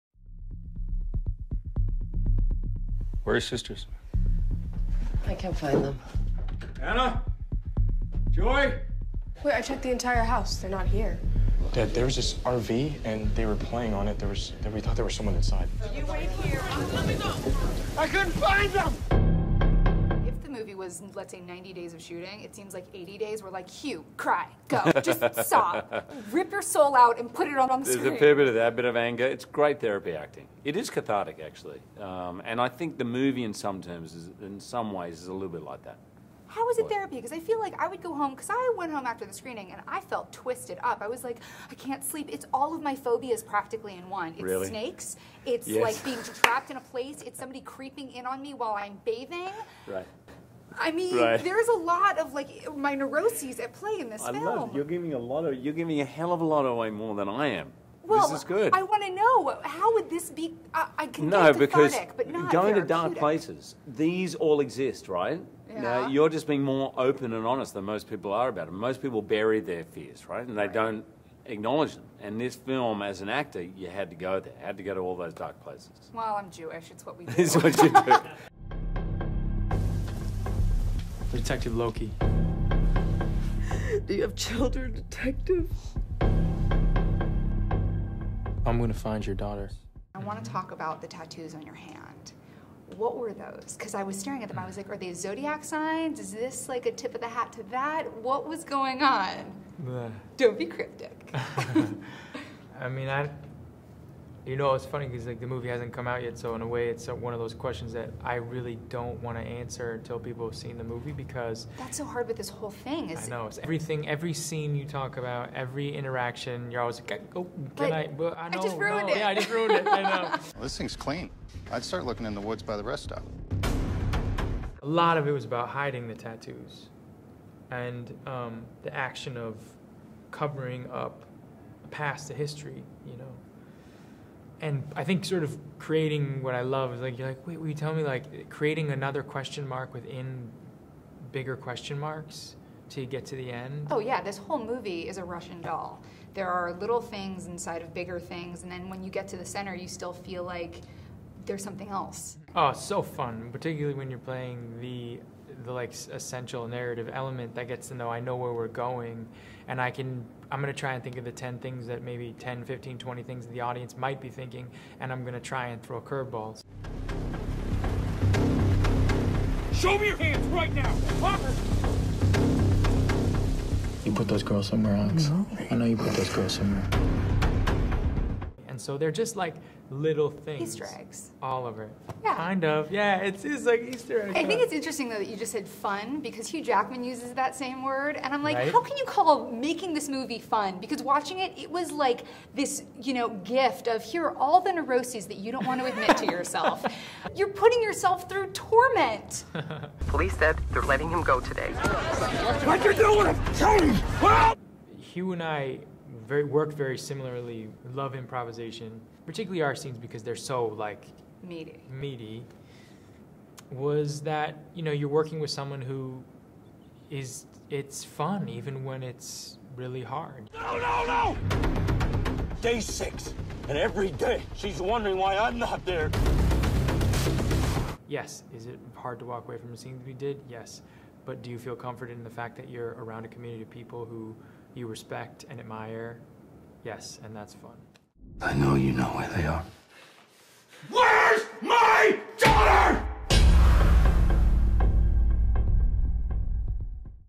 Prisoners Interviews: Hugh Jackman and Jake Gyllenhaal
"Prisoners" stars Hugh Jackman and Jake Gyllenhaal explain why psychological and physical torture is "fun."